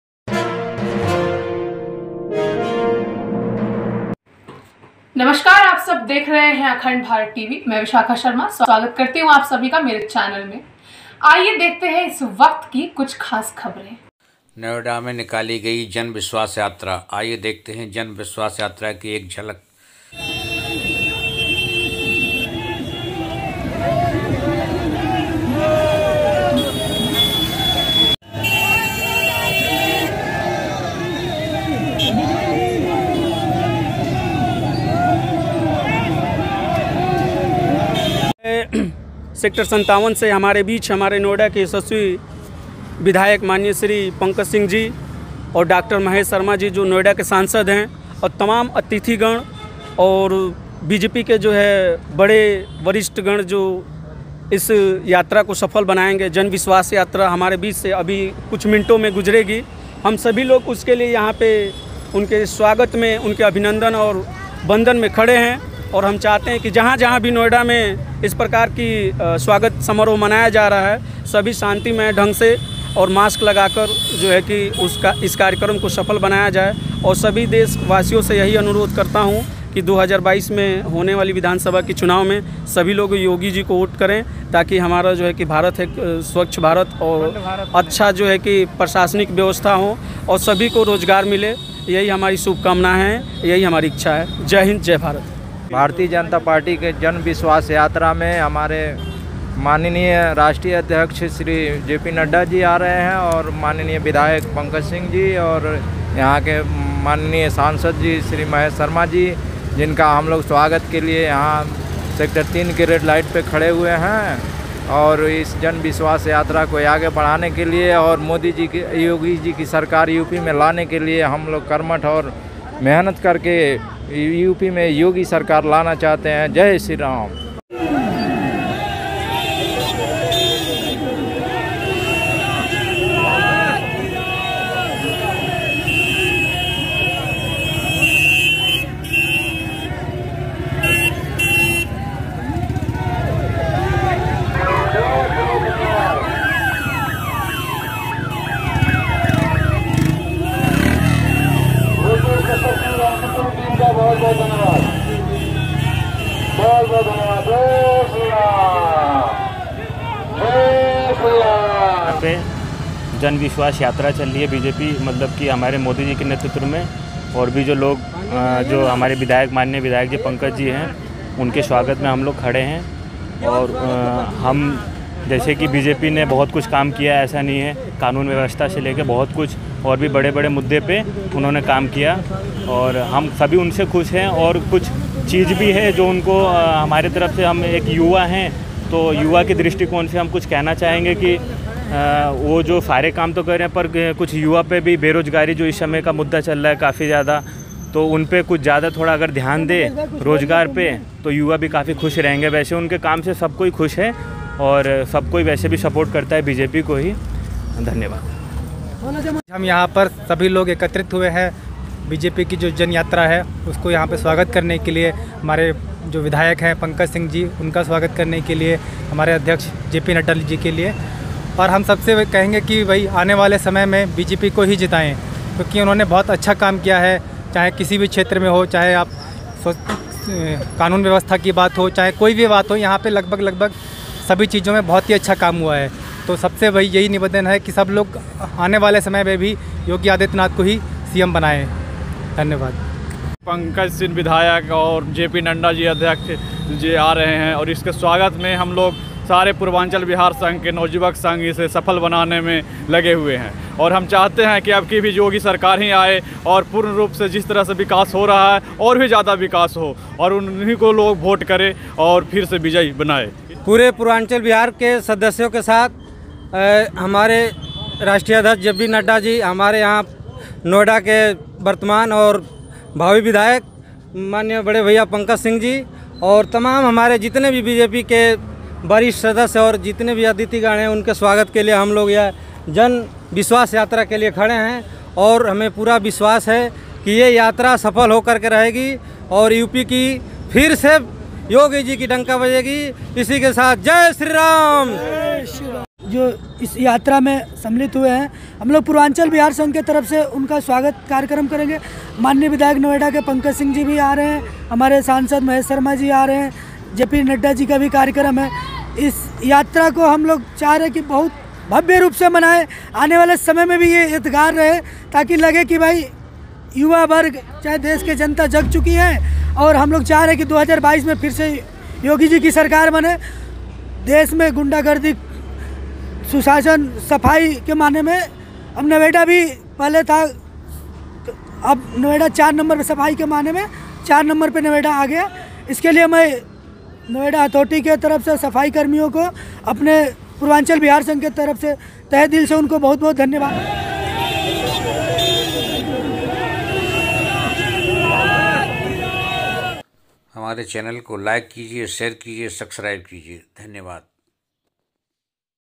नोएडा में जन विश्वास यात्रा के स्वागत में पूर्वांचल विहार संघ के साथ जनसमूह उमड़ा
जन विश्वास यात्रा के स्वागत में नोयडा के पूर्वांचल संघ व जन समूह ने यात्रा का स्वागत किया बीजेपी बड़े बड़े नेता यात्रा में मौजूद थे बीजेपी अध्यक्ष  बीजेपी सांसद बीजेपी विधायक के साथ सभी गणमान्य लोग मौजूद थे

Thank You For Giving Your Time!

Produced By : Akhand Bharat TV
Edit By : Mukul Sharma

अखंडभारत tv
pankaj singh mla